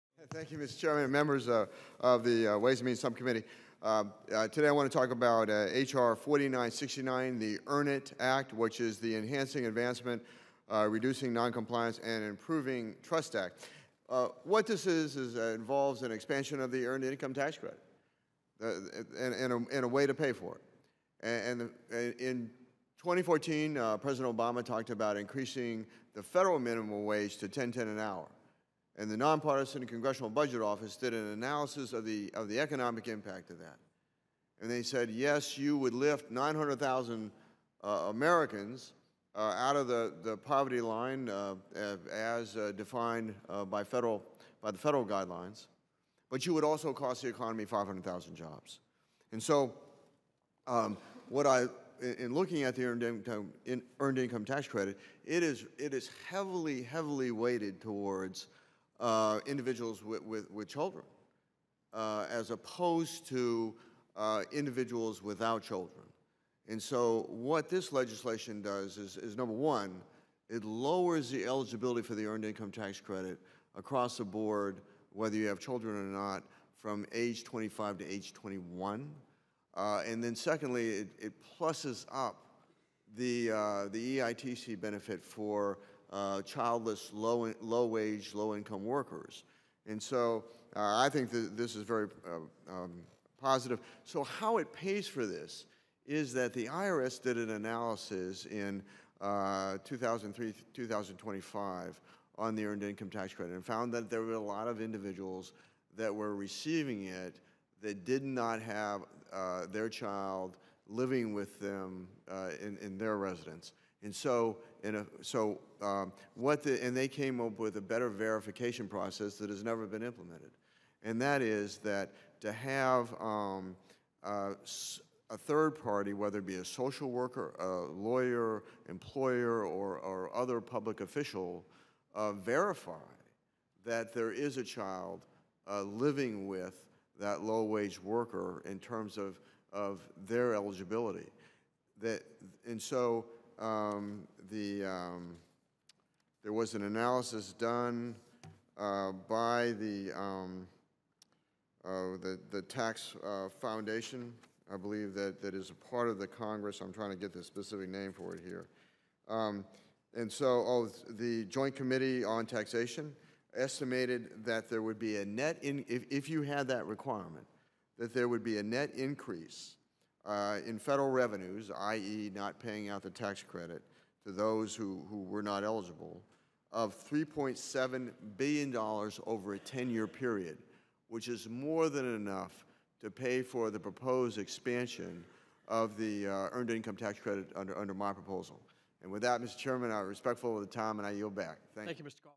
Rep. Coffman Testifies on EARN IT Act
Rep. Coffman testifies on his legislation, the Enhancing Advancement, Reducing Noncompliance, and Improving Trust Act at a Ways and Means Subcommittee Hearing on May 12, 2016.